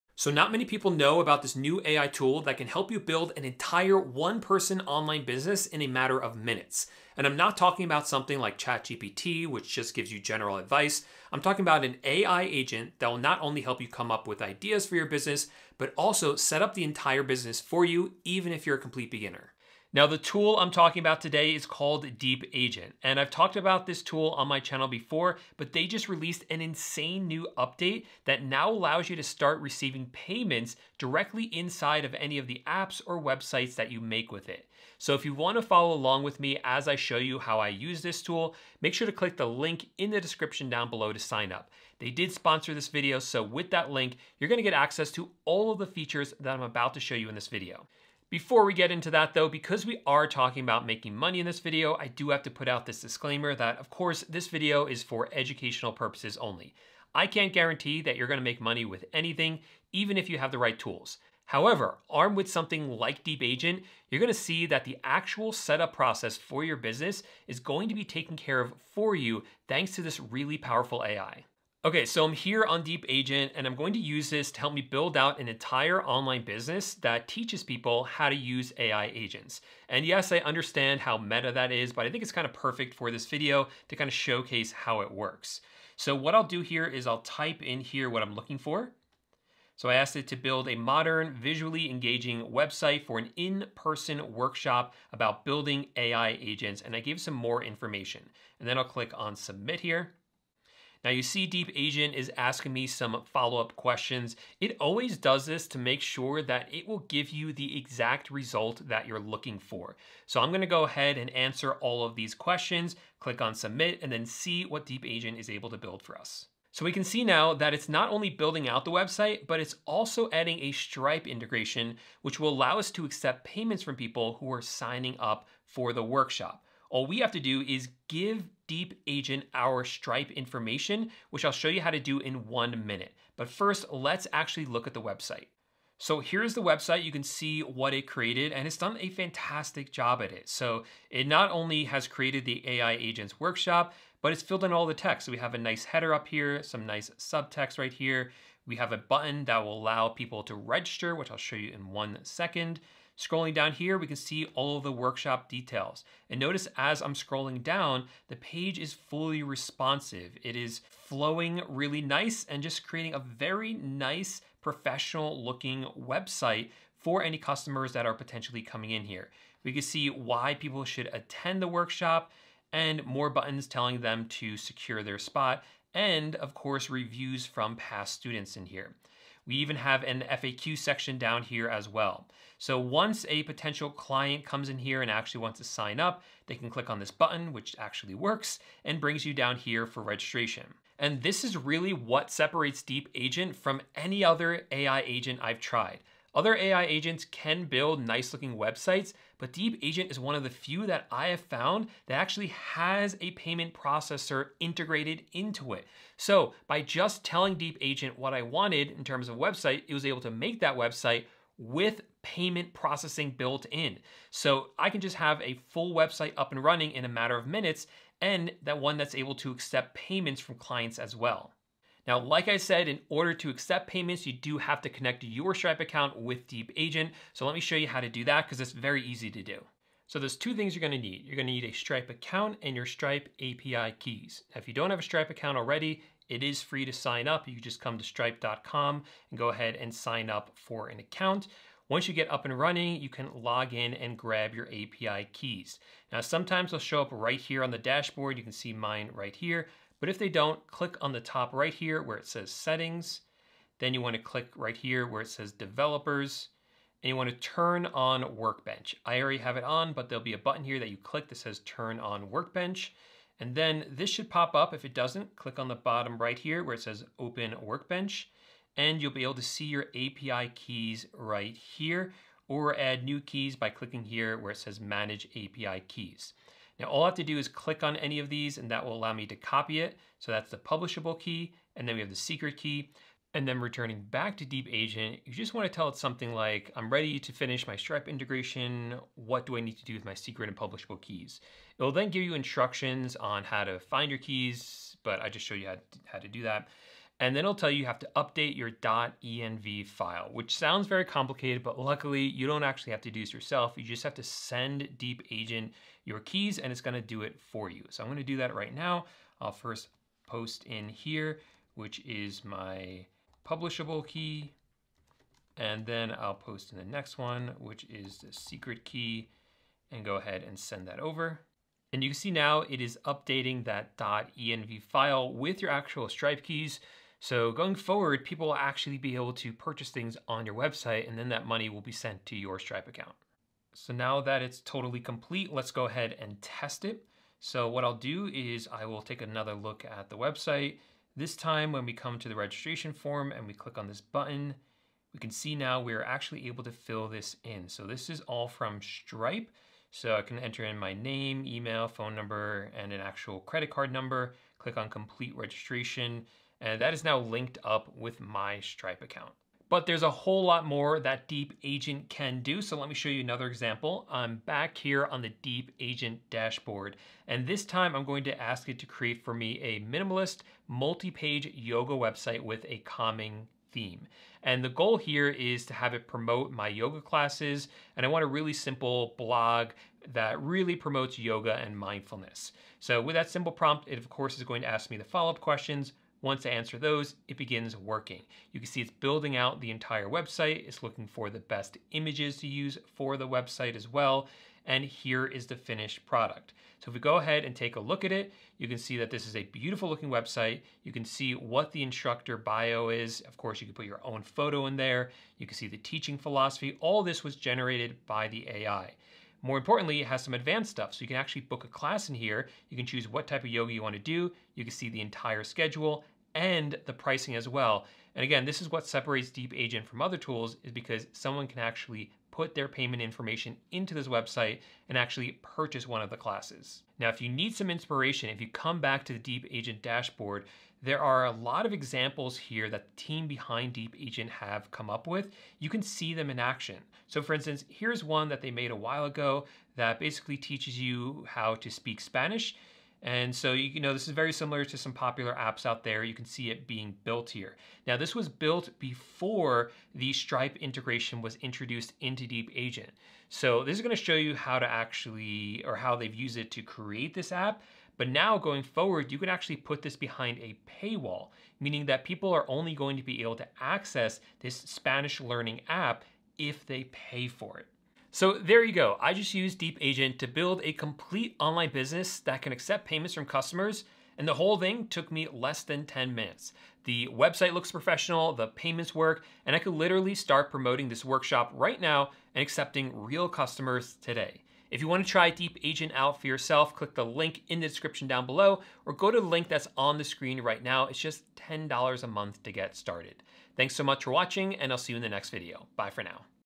This AI Agent BEATS ChatGPT At Building A One Person Online Business!
I just found this insane new AI tool that allows you to create an entire website with payment processing built in, which means that you can start receiving payments from customers in a matter of minutes with this new AI tool.

🤔 Have a question about DeepAgent? Drop it in the comments!